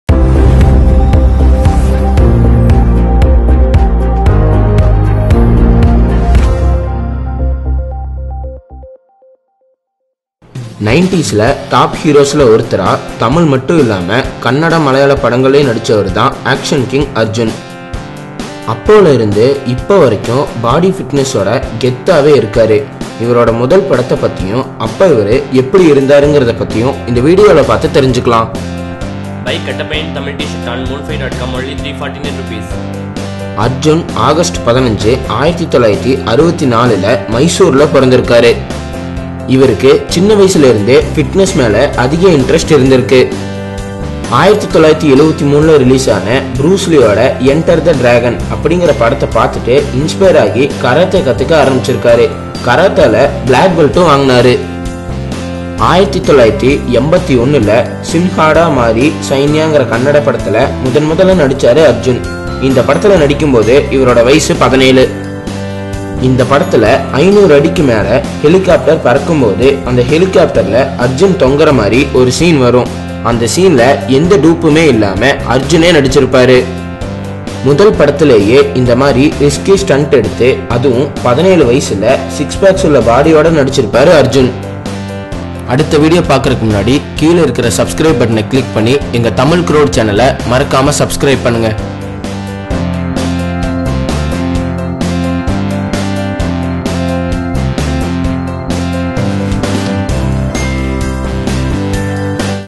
அர்ஜுன் முதல் படம் தெரியுமா? Photos பார்த்தா ஷாக் ஆயிருவீங்க! | Tamil Cinema | Kollywood News
Omg! Is this Arjun? Arjun’s First Film

அர்ஜுன் முதல் படம் தெரியுமா? Photos பார்த்தா ஷாக் ஆயிருவீங்க!

Arjun is an actor, producer, director, and singer who mainly work in Tamil and kannada movies. Here we are going to see about his first film and its rare photos.